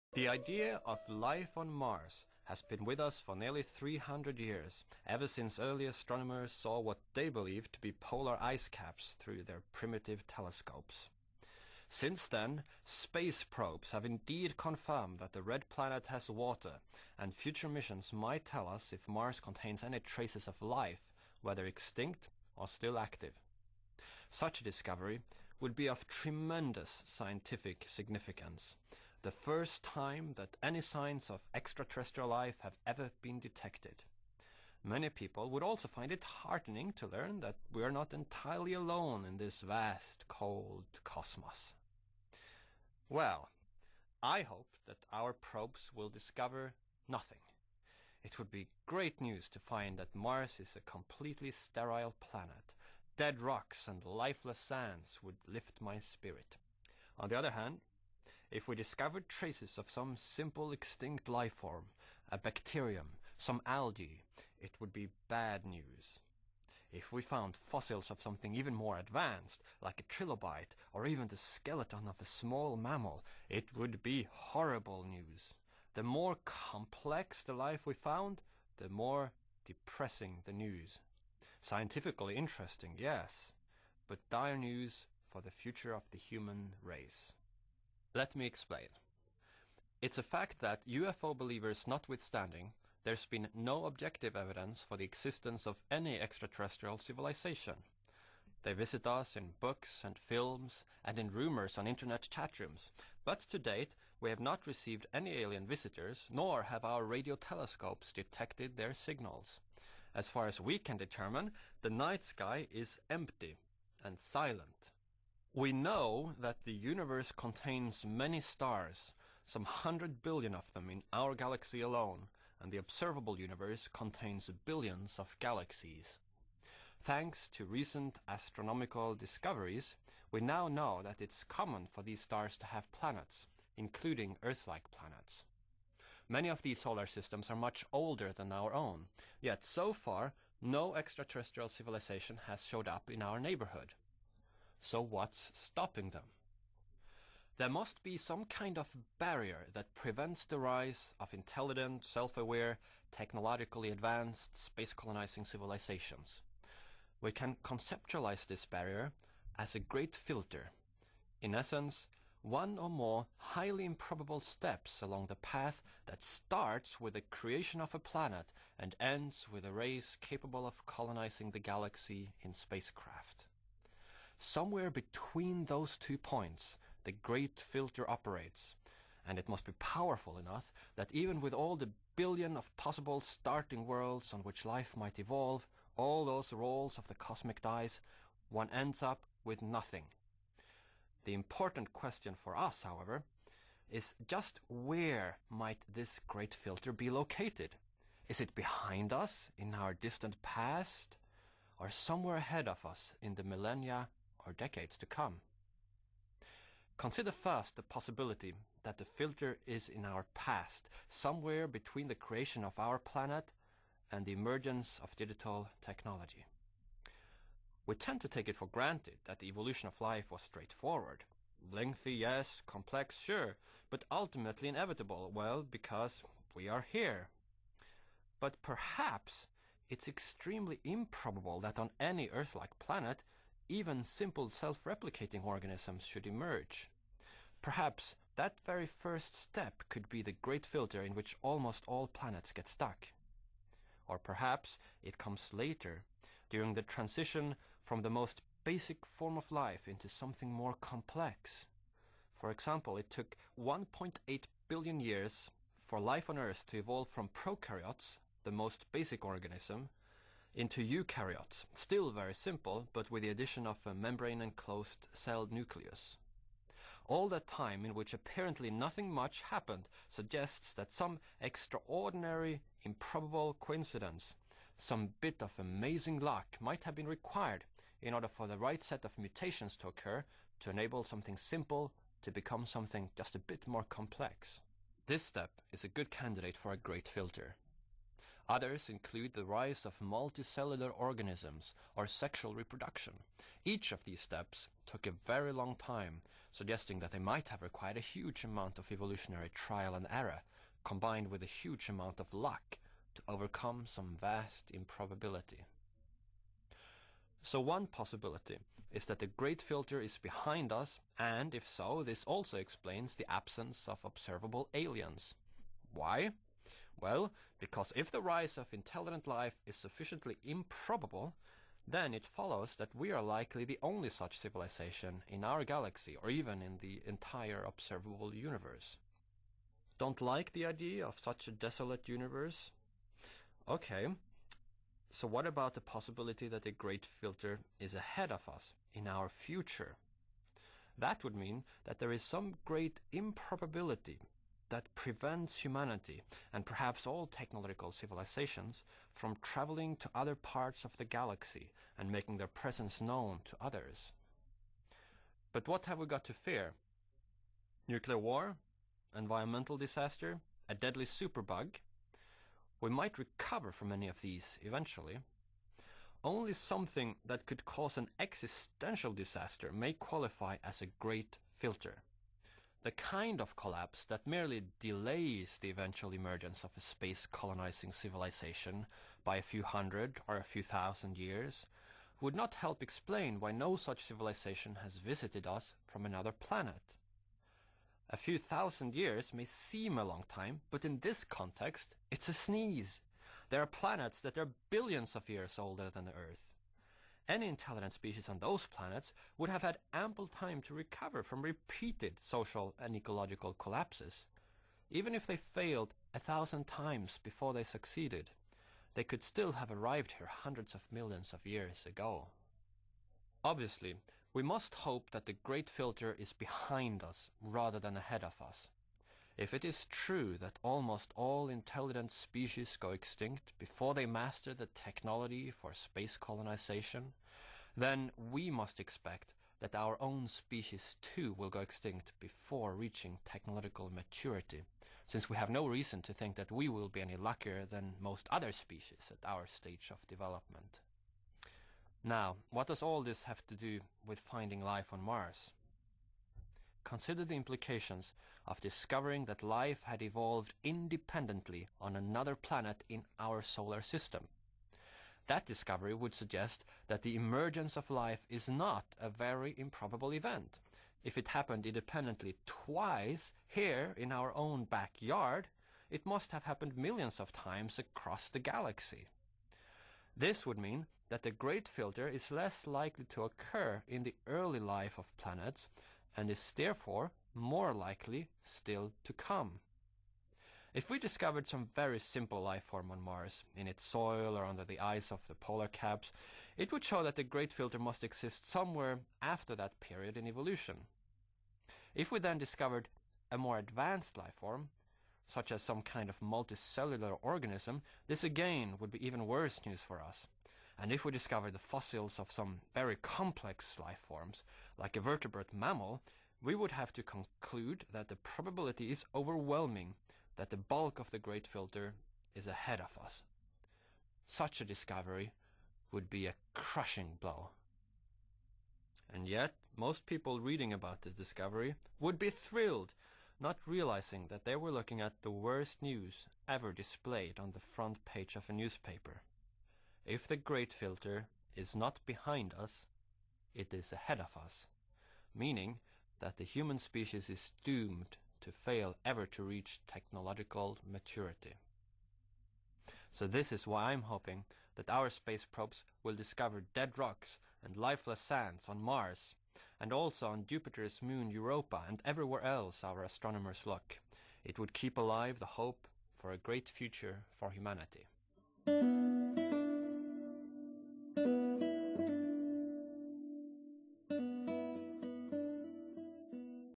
Nick Bostrom on life on Mars - part 1/2
Philosopher Nick Bostrom explains why discovering aliens would be a disaster for humanity.